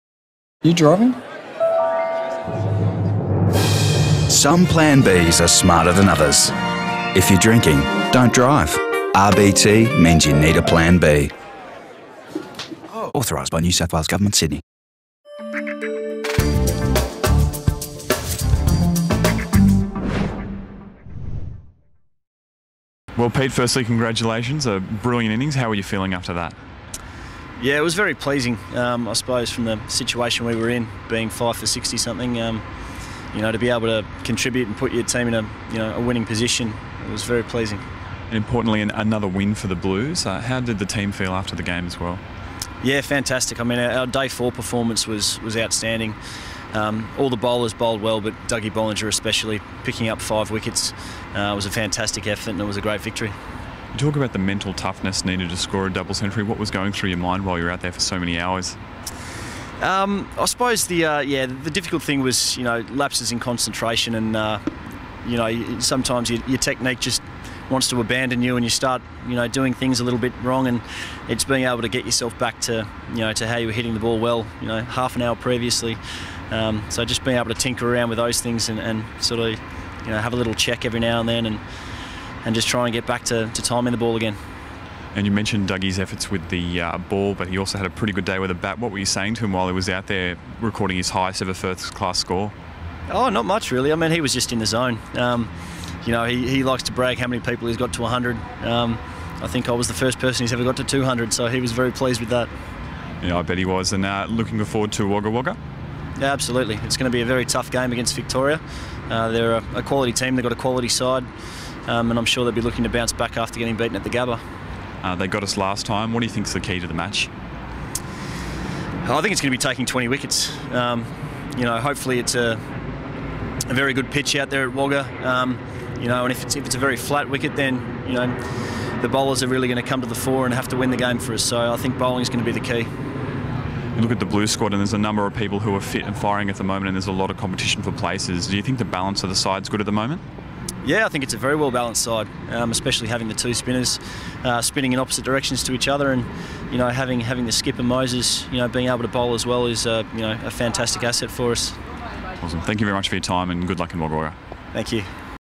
Peter Nevill discusses his Double Century and previews Wagga match
NSW Blues wicket keeper and Double Centurion Peter Nevill caught up with Blues TV to discuss his feats from the match against Tasmania and preview the Blues next clash with the Victorian Bushrangers in Wagga Wagga.